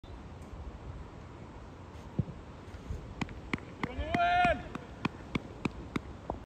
Straffemål Skogås-Trångsund
Hjemmelaget øker til 2-0 på straffe i reservelagskampen Skogås-Trångsund - Sörskogen 3-1.